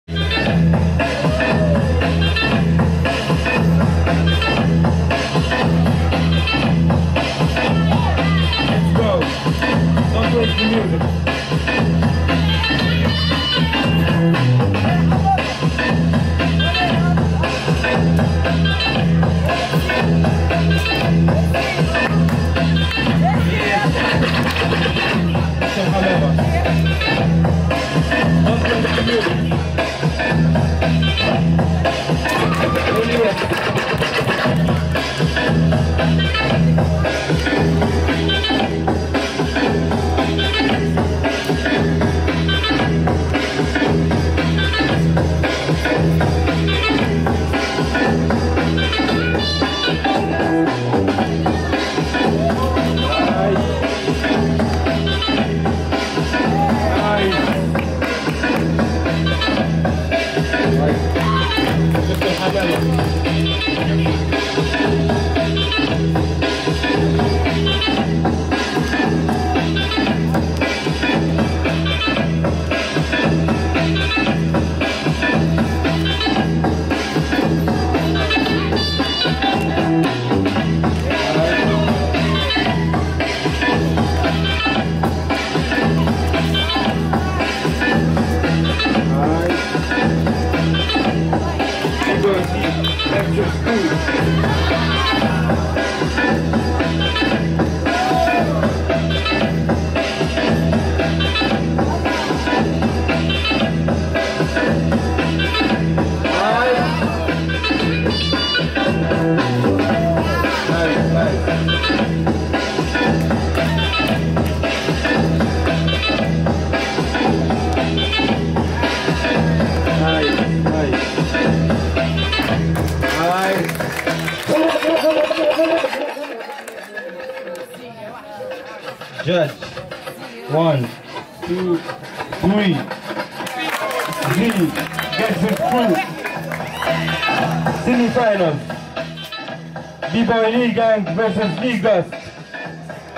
ILLYES vs ZEE vs DR.HAMAMA | Origins | 1vs1 Footwork | Breakreate Festival 2019
August 23, 2019 - Nefza, Beja, Tunisia
21 Sessions: Origins - Breakreate Festival 2019
Winner: ZEE

Judge: South Eagle
DJ: Janmar